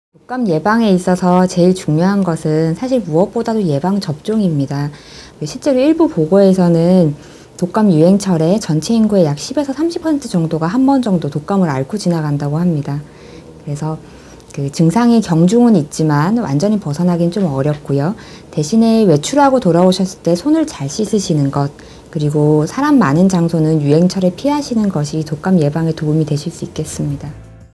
독감 예방에 가장 중요한 것, 예방접종 - (20141105_523회 방송)_겨울철 불청객 독감, 지금부터 예방하세요
[겨울철 불청객 독감, 지금부터 예방하세요]

[ 원고정보 ]
독감 예방에 있어서 제일 중요한 것은 무엇보다도 예방접종입니다. 실제로 일부 보고에서는 독감 유행철에 전체 인구의 약 10~30% 정도가 한 번 정도 독감을 앓고 지나간다고 합니다. 그래서 증상의 경중은 있지만 완전히 벗어나긴 좀 어렵고요. 대신에 외출하고 돌아오셨을 때 손을 잘 씻으시는 것, 그리고 사람 많은 장소는 유행철에 피하시는 것이 독감 예방에 도움이 될 수 있겠습니다.